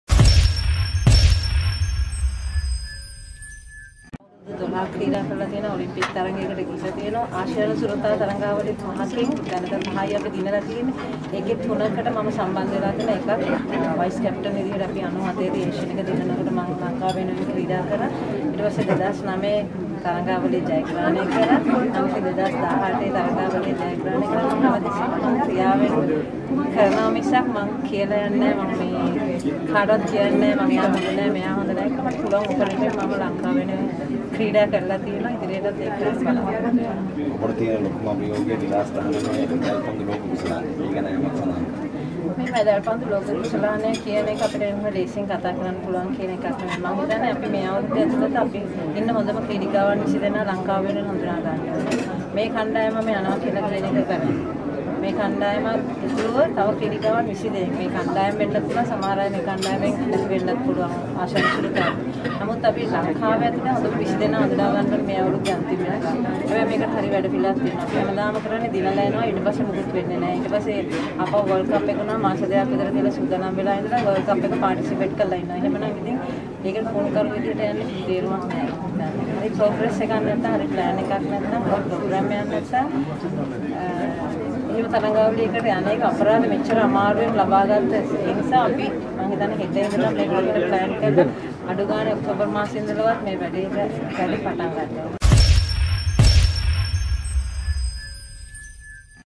Hooraylanka com Thilaka Jinadasa at Airport
මම මගේ පොරොන්දු ක්‍රියාවෙන් ඔප්පු කරන කෙනෙක් මිසක් , තැන තැන කියව කිය ඉන්න කෙනෙක් නෙවේ. ලෝක කුසලාන තරඟාවලියට යන්න ඉතා හොඳ පුහුණුවීමක් අවශ්‍යයි - ශ්‍රී ලංකා දැල්පන්දු පුහුණුකාරිනිය